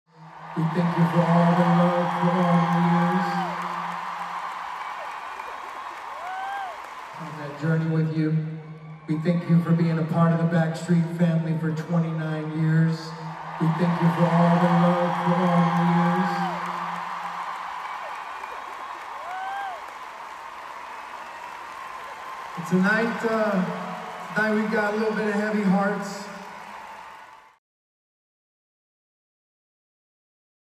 Backstreet Boys' Nick Carter cries as band pays tribute to singer's brother Aaron
The Backstreet Boys’ Nick Carter was visibly emotional on stage at London’s O2 Arena on Sunday, 6 November, as the band paid tribute to his brother Aaron Carter who was found dead at home on Saturday.

The US musician, 34, opened for his elder brother’s band numerous times.

Nick’s bandmates hugged him as the group took a moment to remember his brother.

“I will miss my brother more than anyone will ever know. I love you Chizz. Now you can finally have the peace you could never find here on earth,” Nick wrote in an Instagram post.